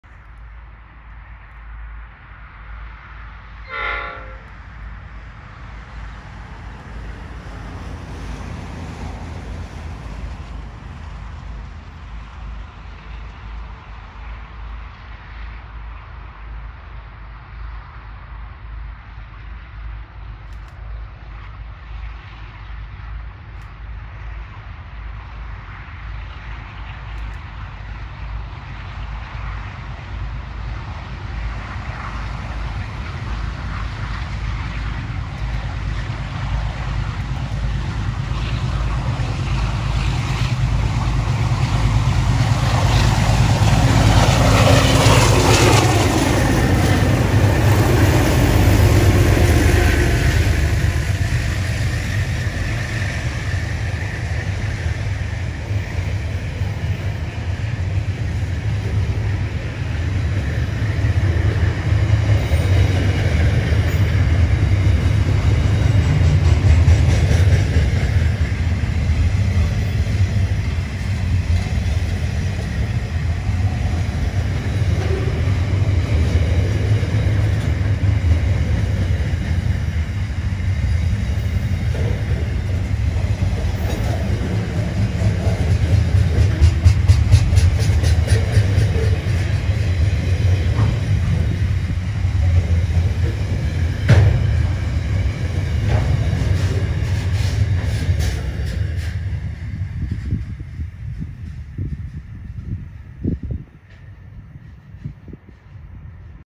QUBE Harefield Shuttle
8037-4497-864 on 3316 Harefield to Junee Shuttle on Tuesday 9/6/2020